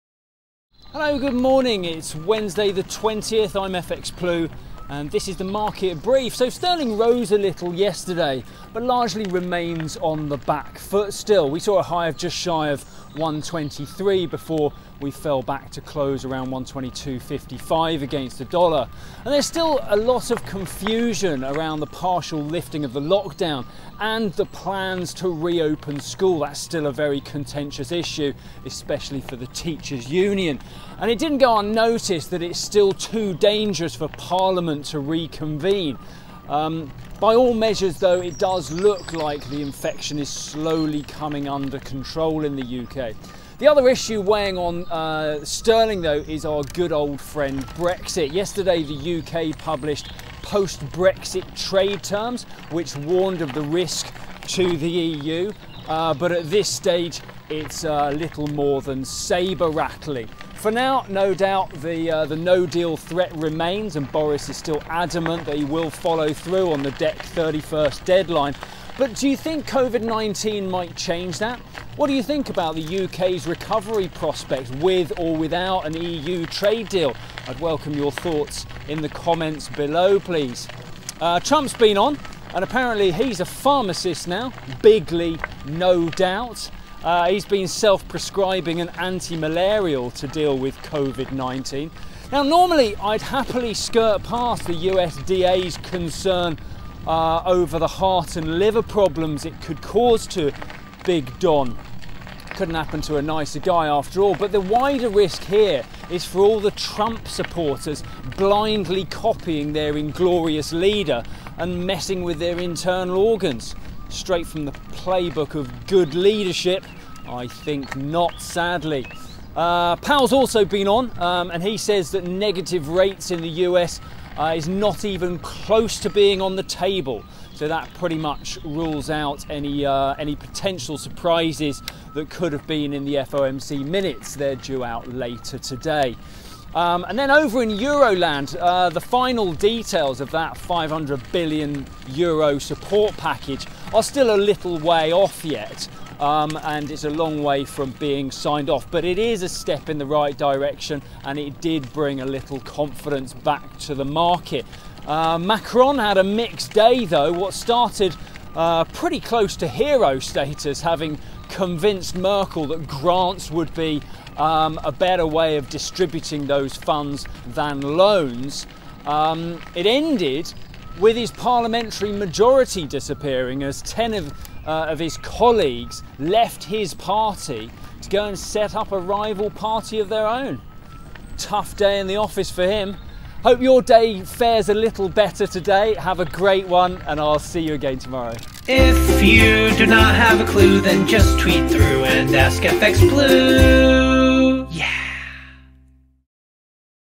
Sterling clings to gains | FX News | 20 May 2020
UK sets out its post Brexit trade terms
Sterling remains under pressure and is still the worst performing currency in the G10 this month, despite two reasonably strong days so far this week.

The confusion over the partial lifting of the lockdown, the reopening of schools and the ability of the Government to get funds to those who most need it, is more a testimony to the return of industrial relations woes and the ineptitude of Boris Johnson’s newer recruits than a concern over the spread of Covid-19.